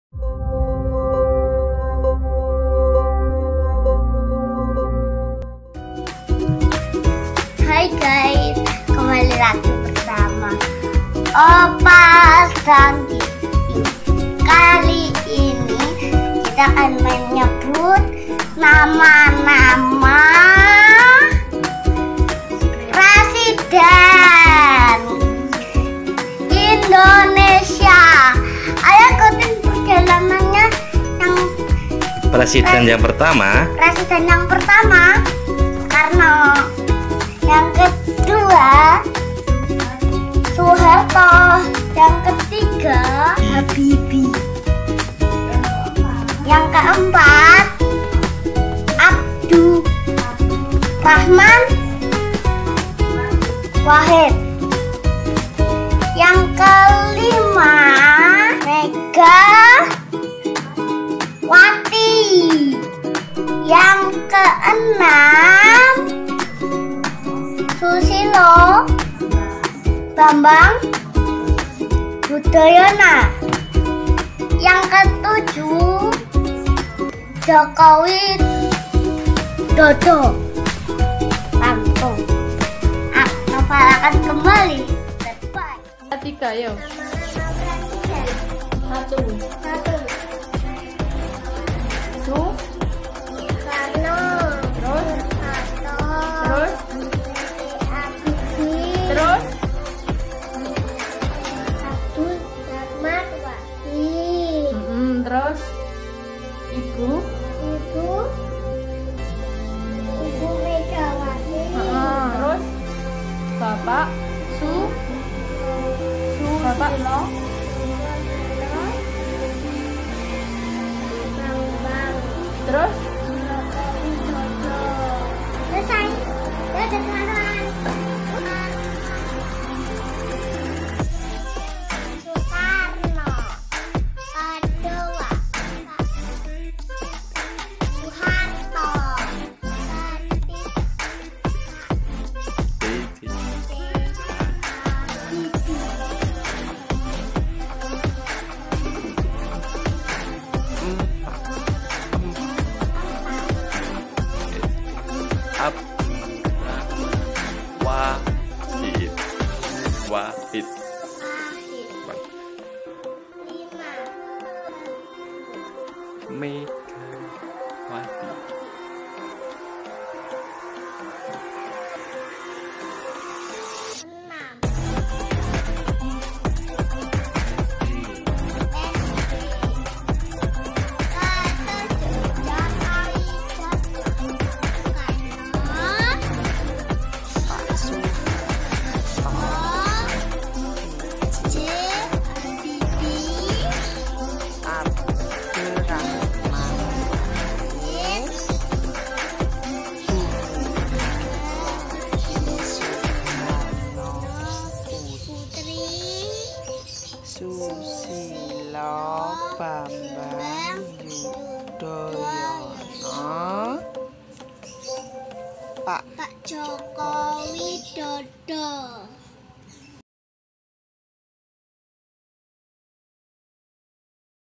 Mengenal nama Presiden Indonesia Kegiatan PAUD/TK tema Negaraku - kemerdekaan - Tanah Airku
Hallo Ananda :)  Kembali bertemu dengan kami Anak Indonesia. Kami dari TK ABA Bina Insani ingin mempersembahkan aktivitas kami selama lockdown yaitu belajar di rumah. Kali ini kita dapat tugas menyebutkan nama2 pemimpin Indonesia yang kami ketahui. Siapa aja ya nama nama pemimpin itu. Terimakasih banyak :)